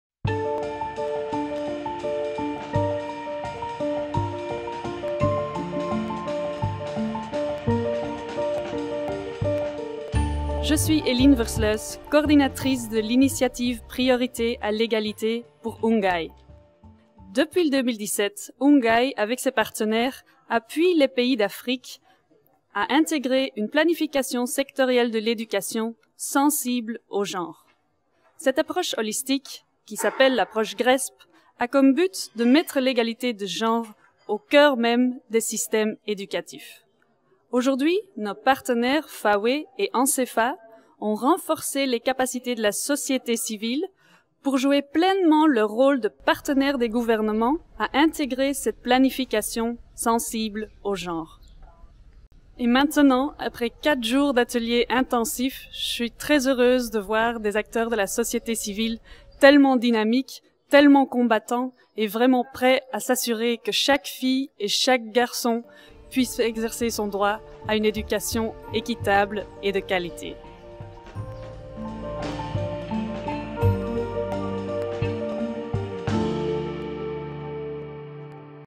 Eline Versluys - GRESP workshop Dakar, Senegal, 2021
Coordinator of the Gender at the Centre Initiative, Eline, on the importance of partnership to place gender equality at the heart of education systems.